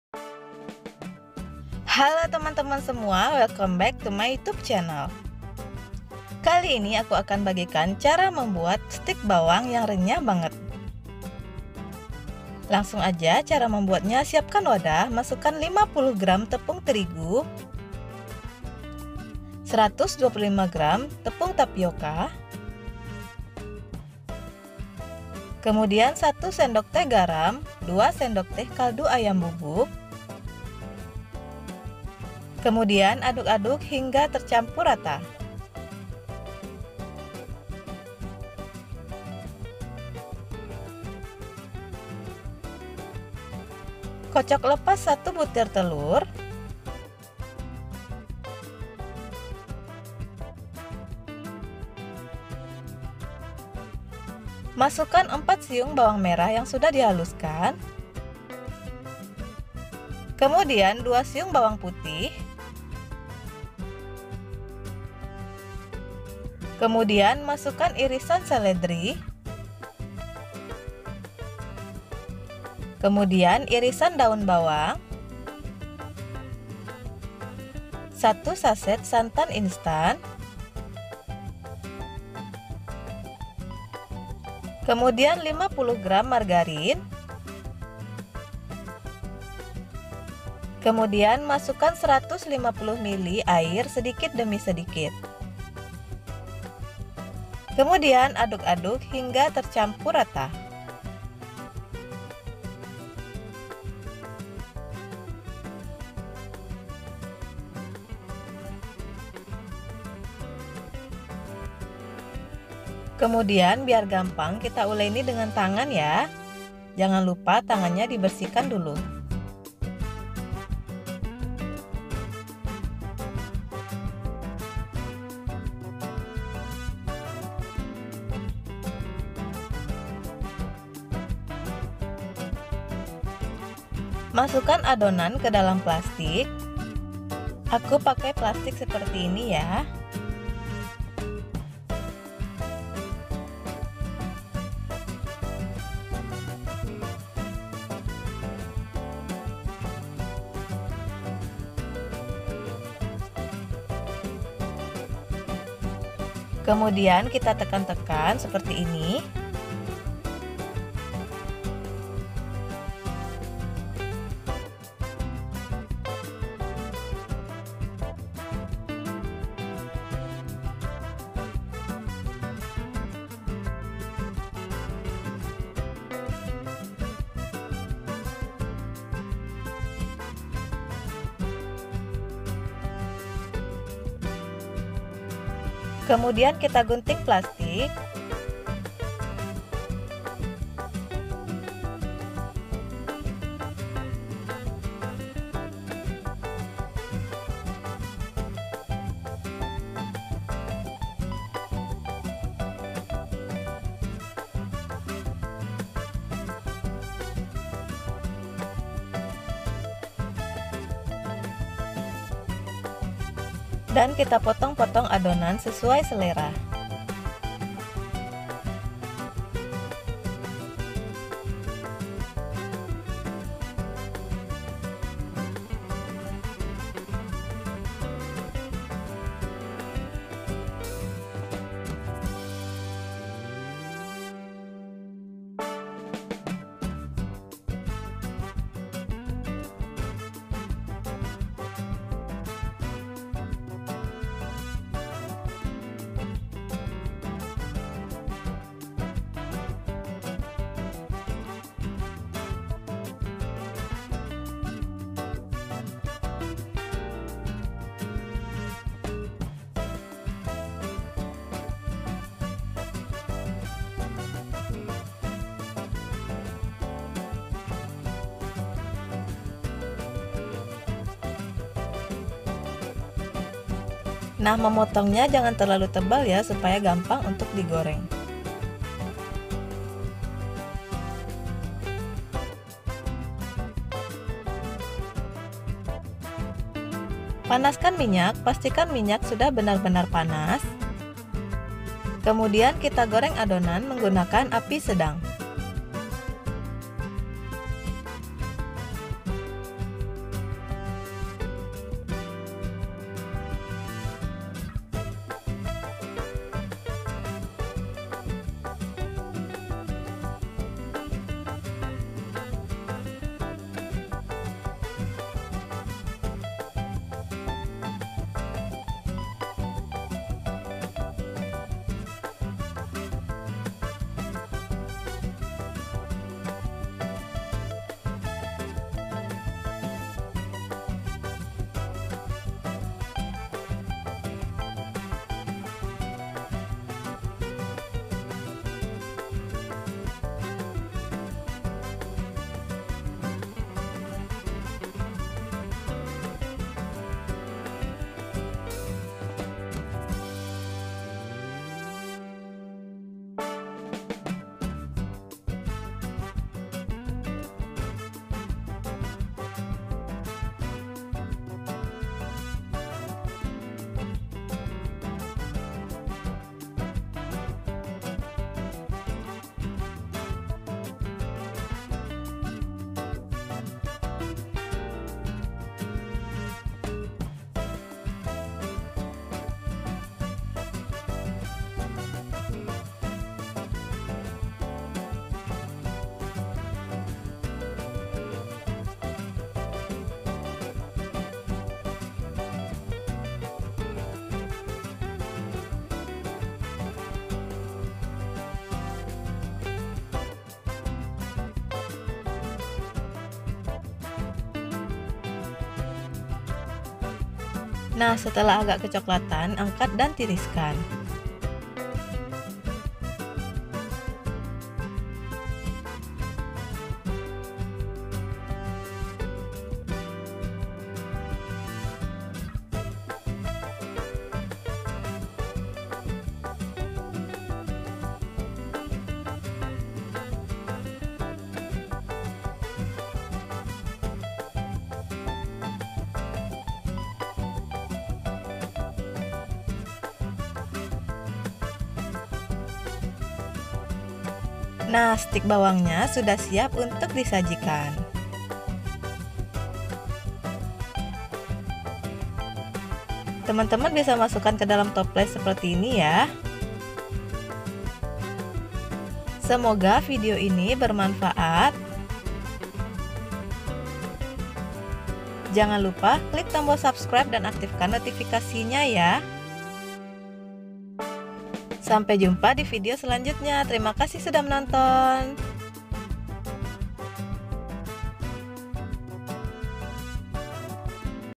CUMA DIADUK STIK BAWANG MERAH SUPER RENYAH DAN GAMPANG I CEMILAN LEBARAN
kali ini aku akan bagikan RESEP STIK BAWANG RENYAH CEMILAN LEBARAN

Bahan sistik bawang :

500 gr tepung terigu (segitiga biru)
125 gr tepung tapioka / kanji (rose bran)
50 gr margarin (blue band)
150 ml air
1 saset santan instan (kara)
1 butir telur
Daun bawang secukupnya (diiris tipis)
Seledri secukupnya (diiris tipis)
4 siung bawang merah (dihaluskan)
2 siung bawang putih (dihaluskan)
2 sdt kaldu ayam bubuk
1 sdt garam

tonton videonya hingga selesai
like dan tinggalkan komentar